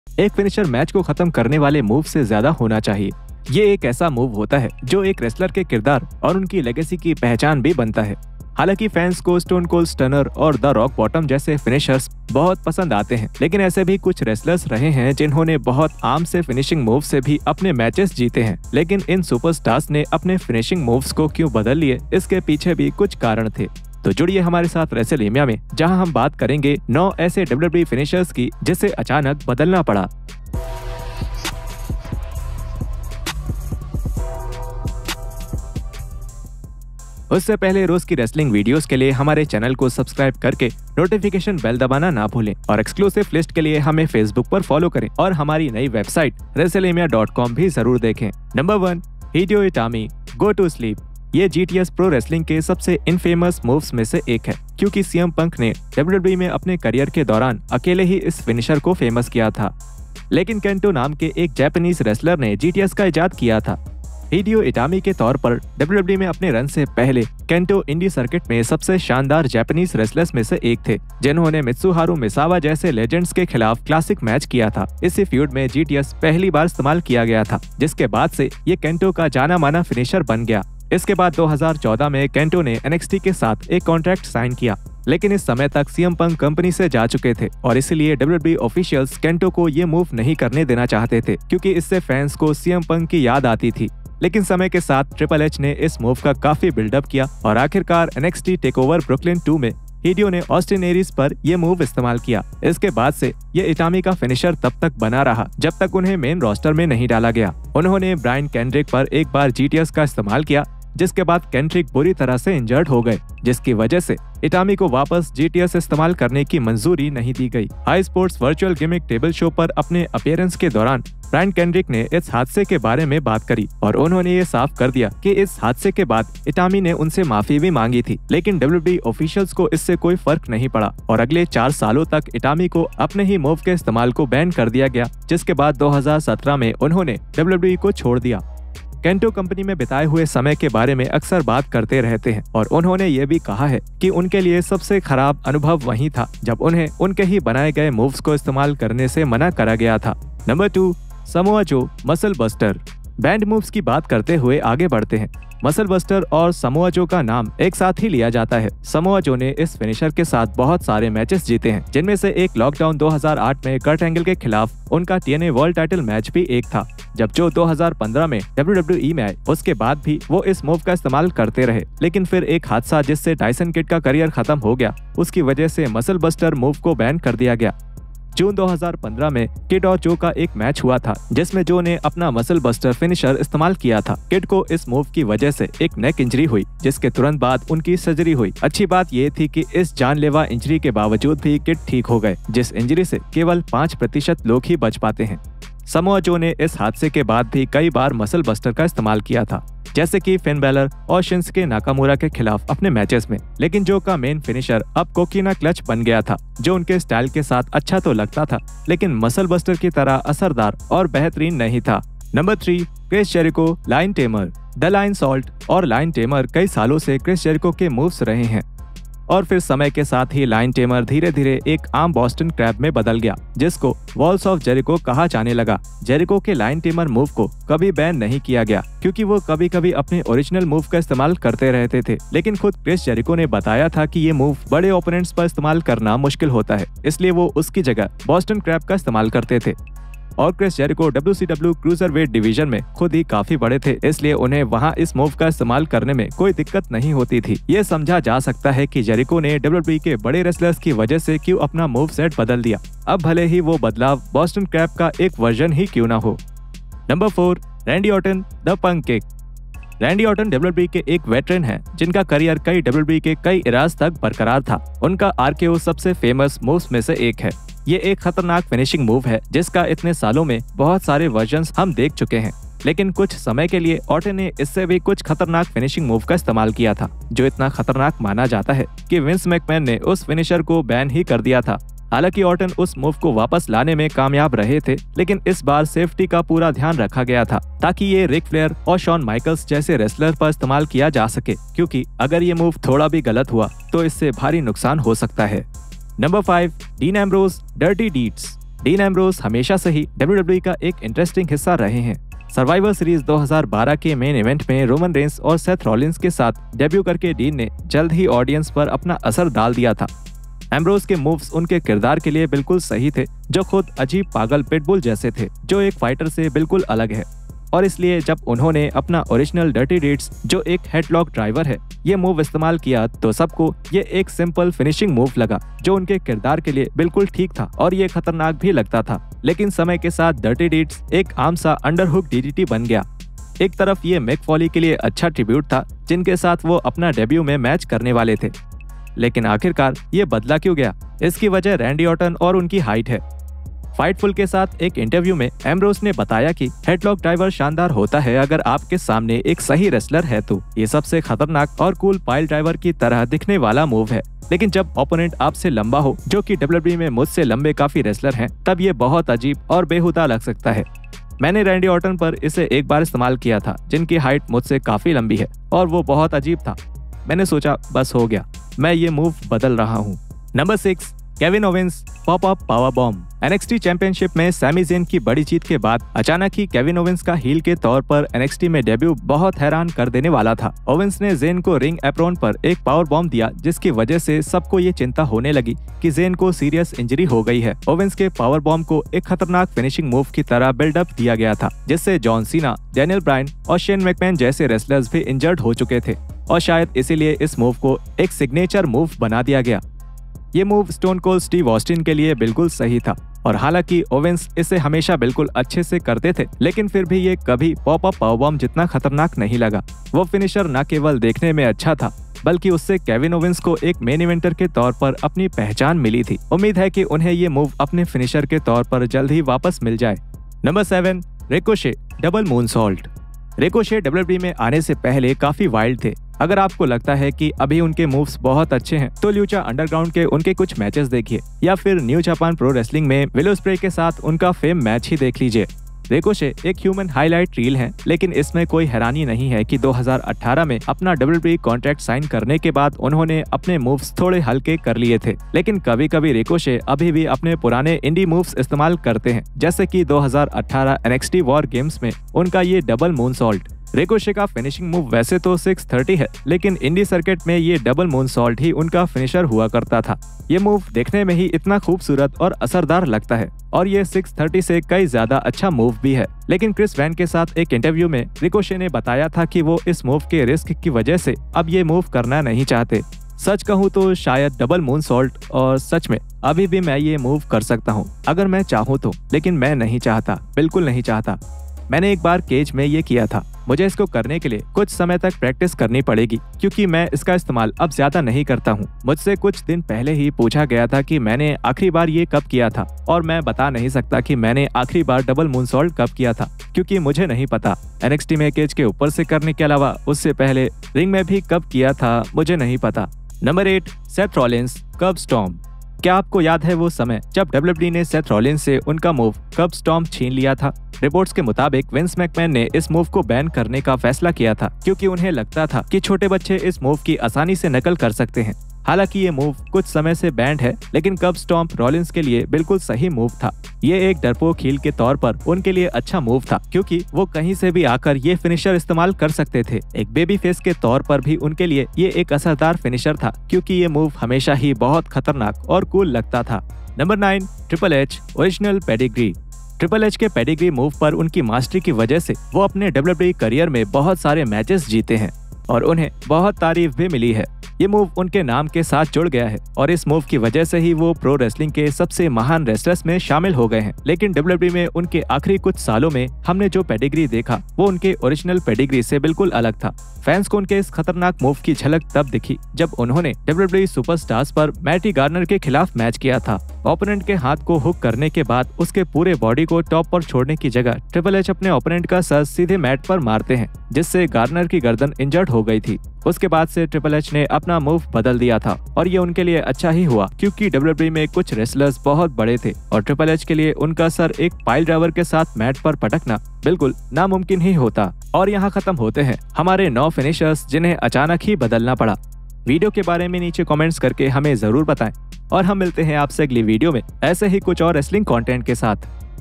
WWE Finishers जिन्हे तुरंत change करना पड़ा
WWE Superstars Jinhe Apna Wrestling Finisher Change Karne Ke Liye Force Kiya Gaya

Topics Discussed In this Video:

1. WWE Finishers That Had to Be Instantly Changed
2. WWE Finishers That Were Too Dangerous
3. WWE Wrestlers Who Changed Their Finisher After Injuring a Wrestler
4. WWE Superstars who were told to change their finisher
5. Wrestlelamia हिन्दी